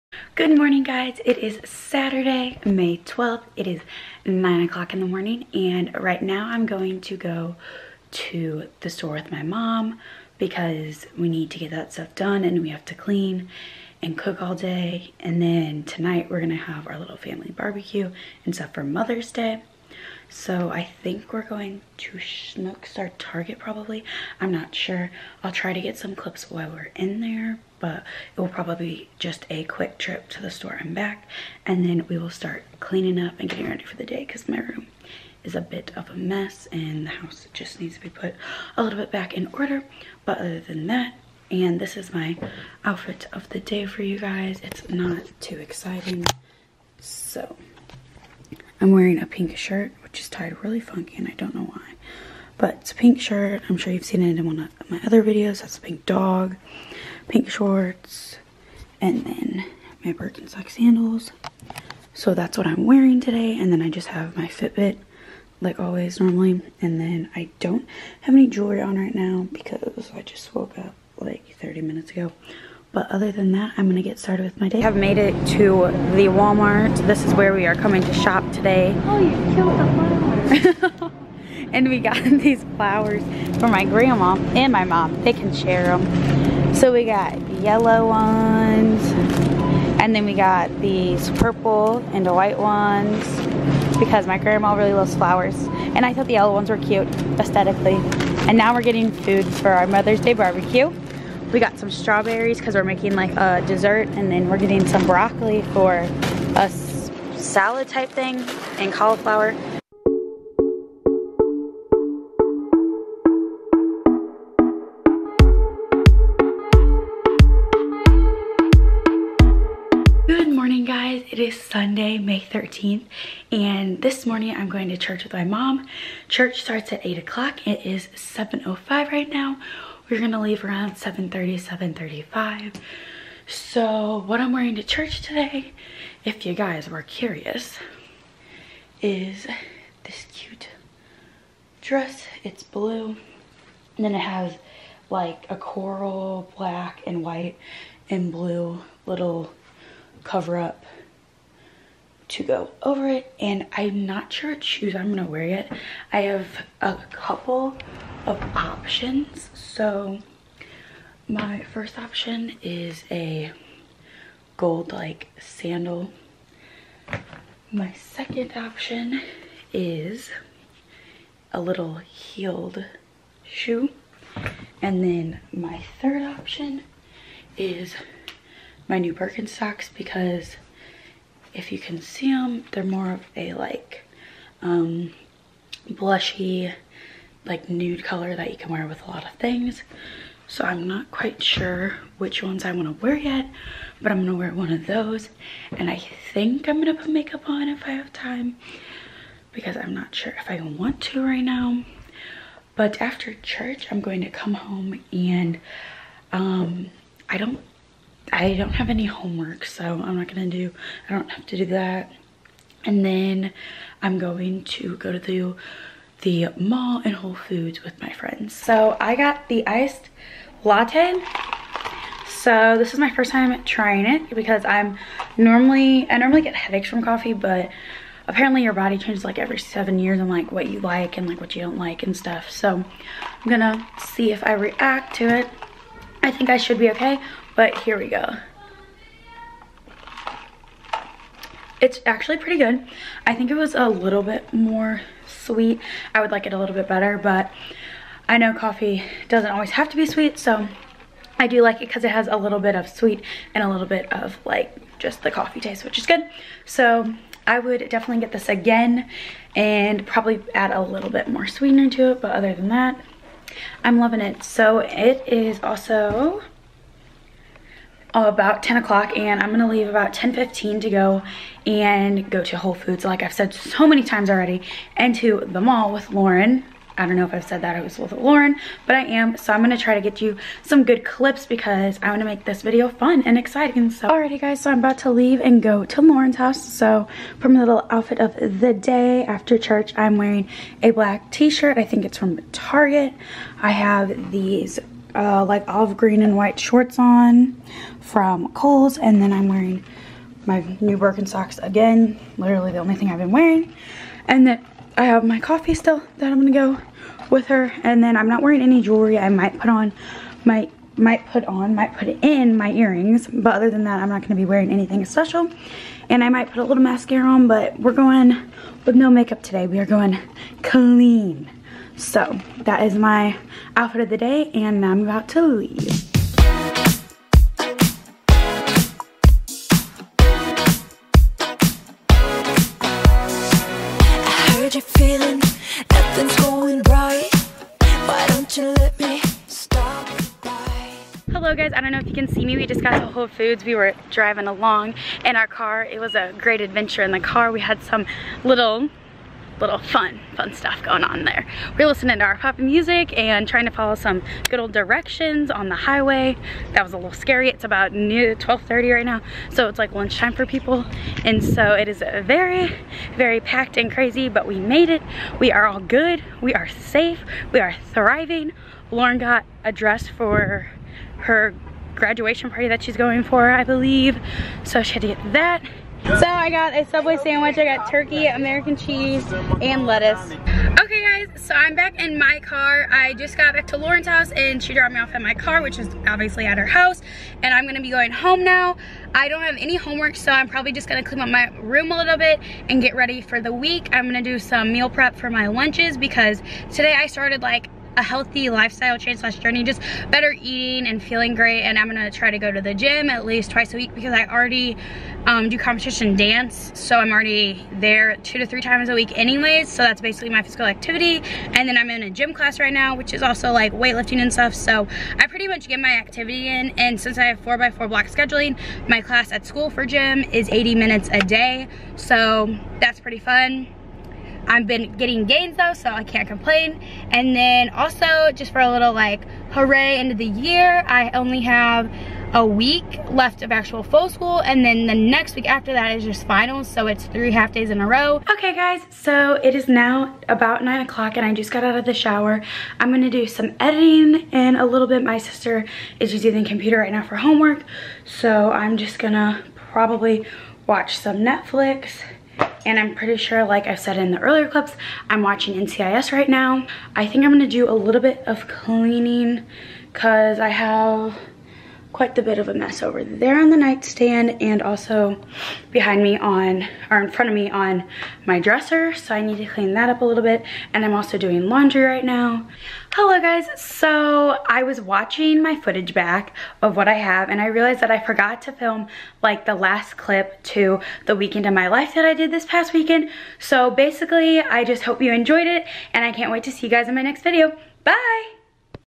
Weekend in My Life
In this weeks video I vlogged a weekend in my life. I got to hang out with my friend and do some shopping. I hope you all enjoy this weekend in my life and I can't wait to see you in my next video!